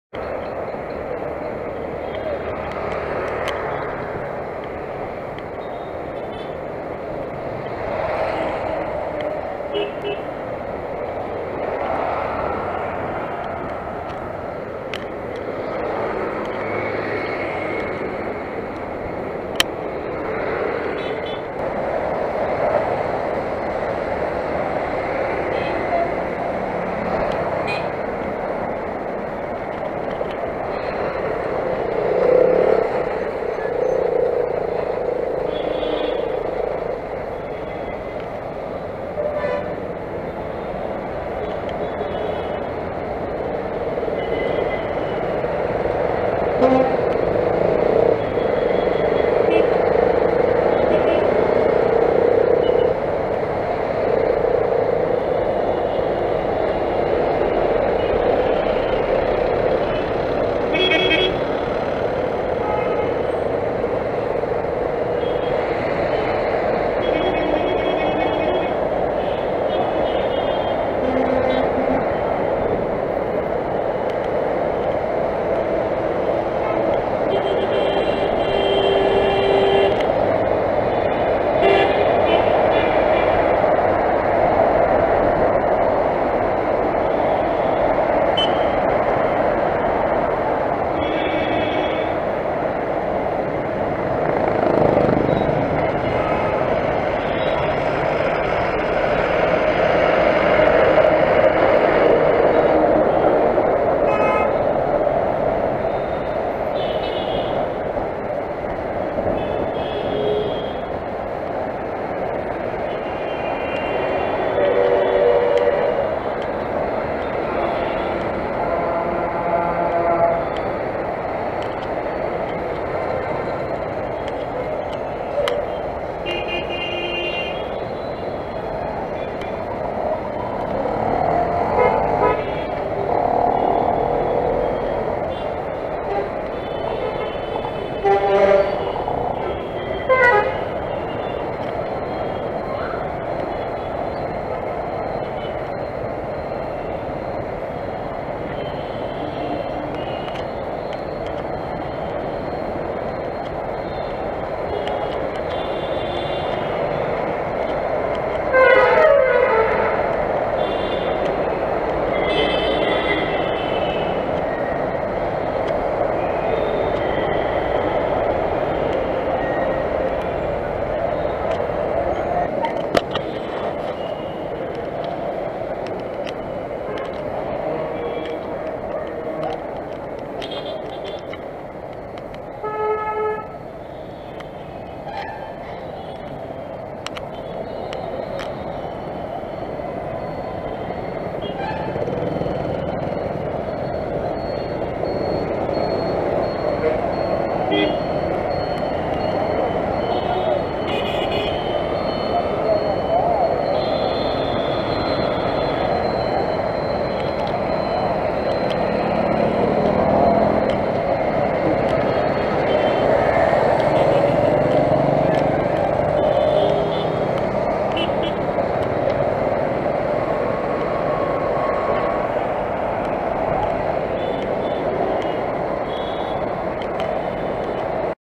Cycling - Chennai Traffic | Close calls highlights
Few clips from 28th Oct Ride. Tambaram - Guindy - OMR - Home